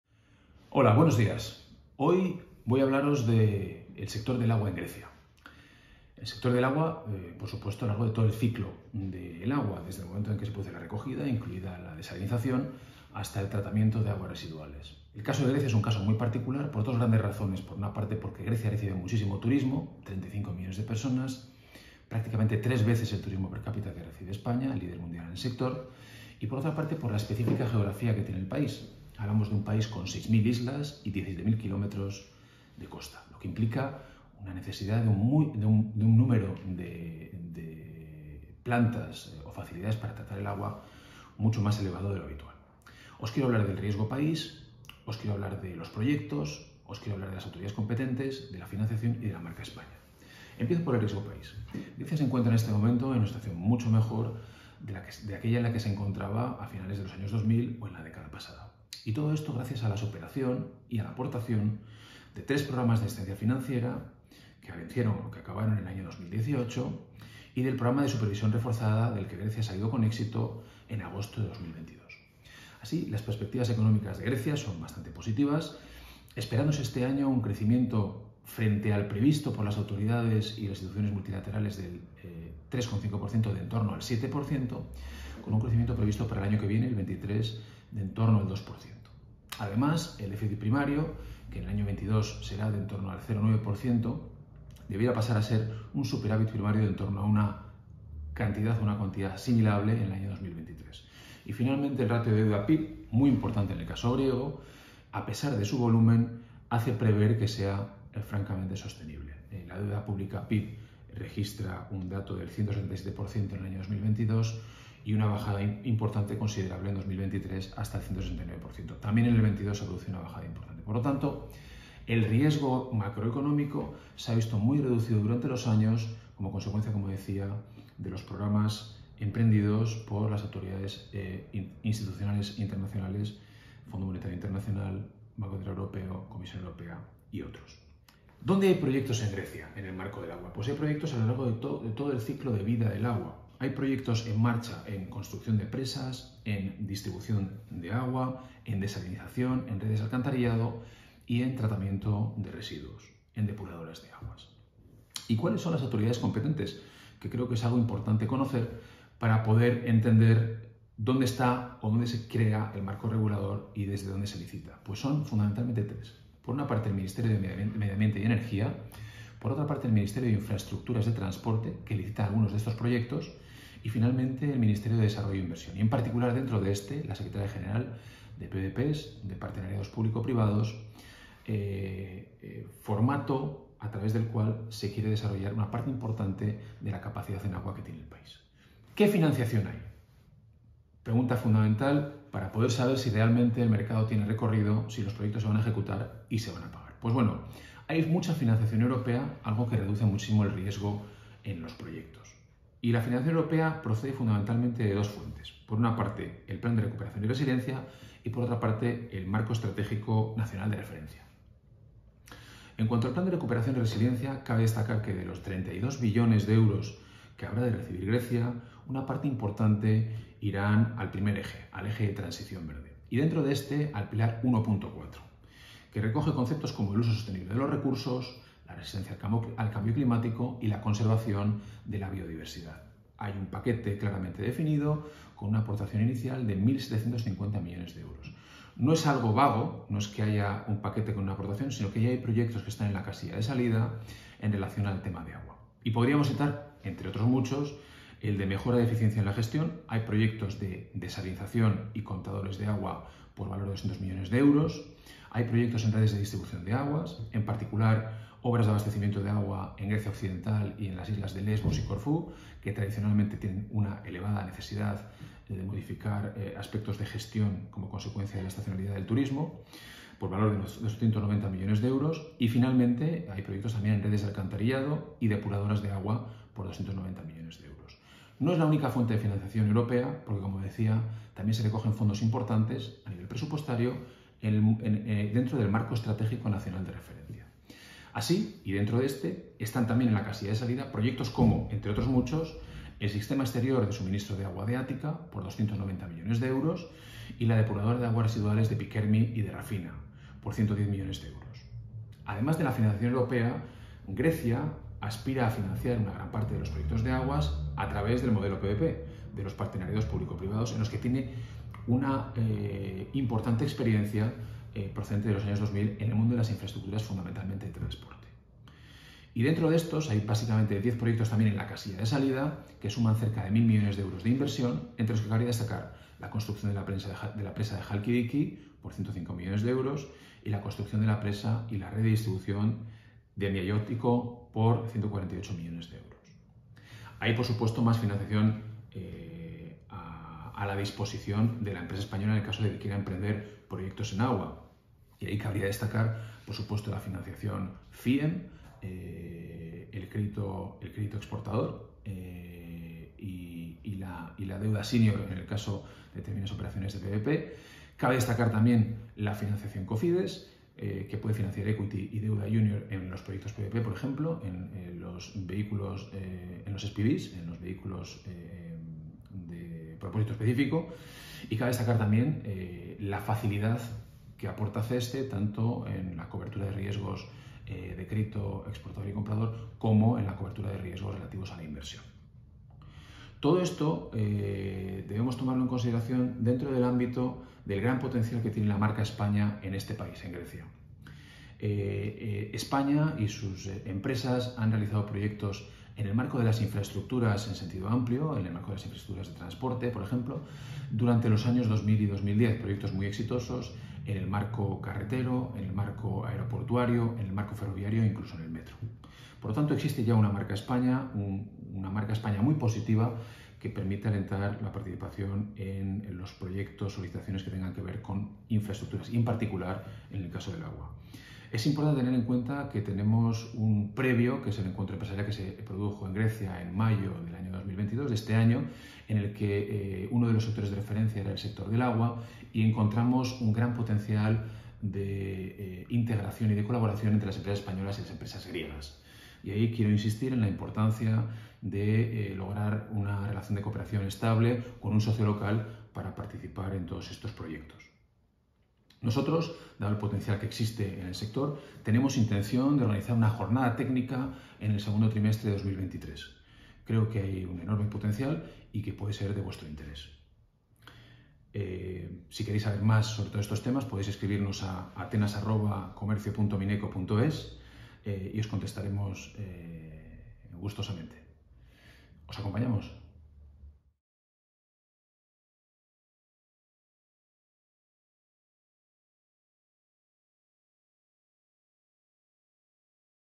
Conoce las oportunidades de negocio e inversión en el sector del agua en Grecia
Jorge Andreu Arasa, Consejero económico y comercial de España en Grecia, describe las diversas oportunidades que el país heleno presenta en el ámbito del agua en todo su ciclo y desgrana algunos de los proyectos más atractivos.

Toda la información, en este vídeo.